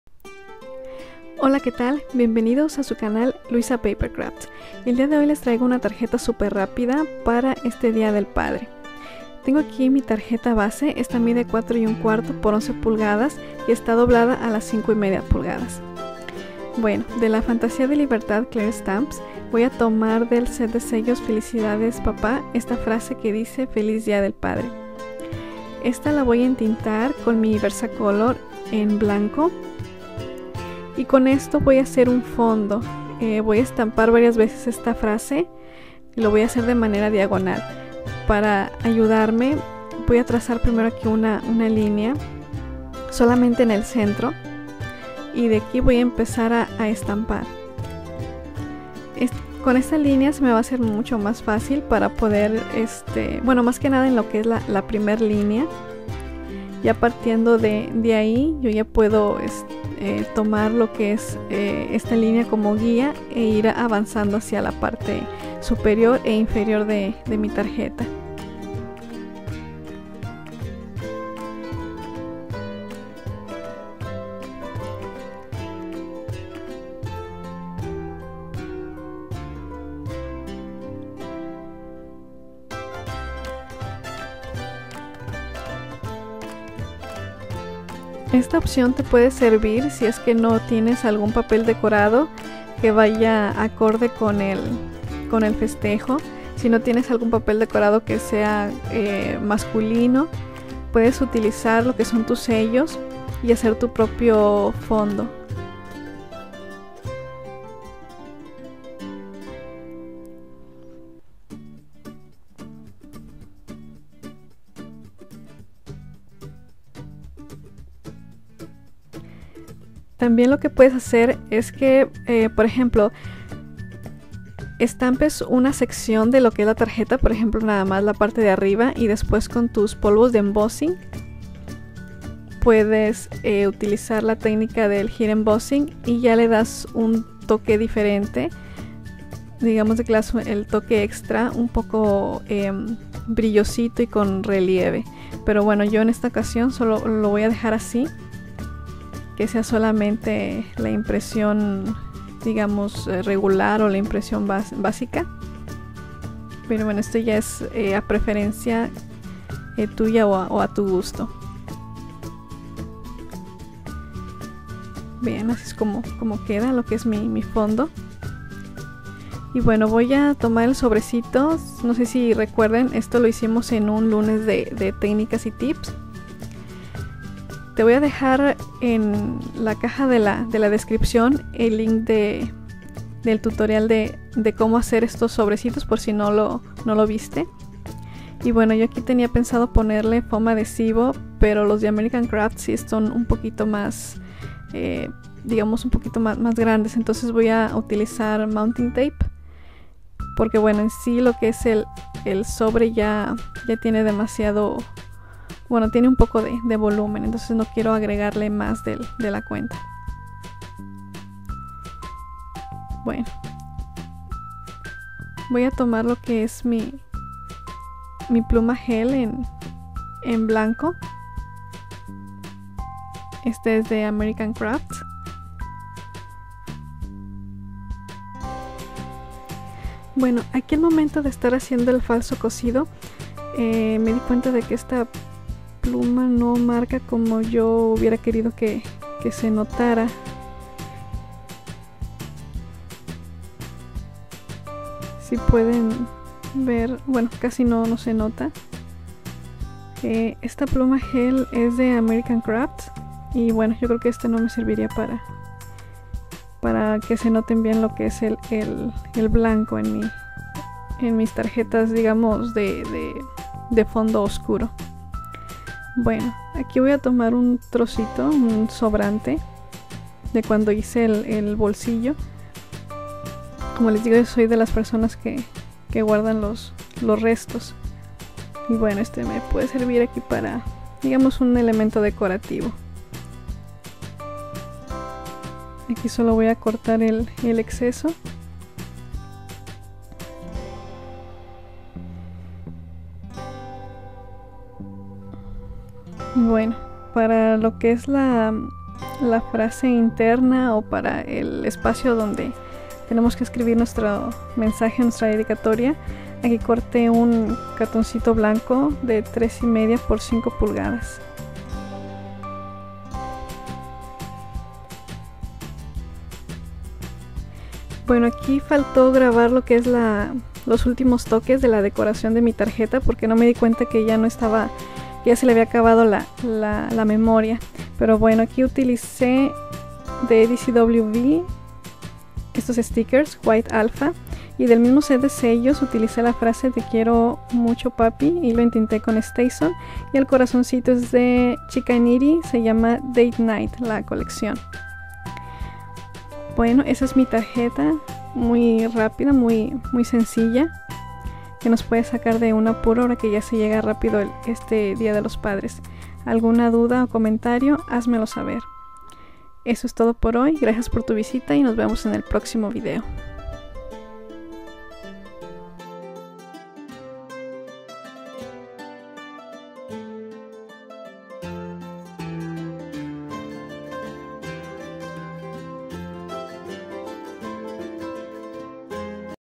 Tutorial Tarjeta Día del Padre
Tutorial de otra tarjeta simple y rápida para el Día del Padre usando la técnica de Heat Embossing o Embosado caliente. Para mas detalles visita esta entrada en mi blog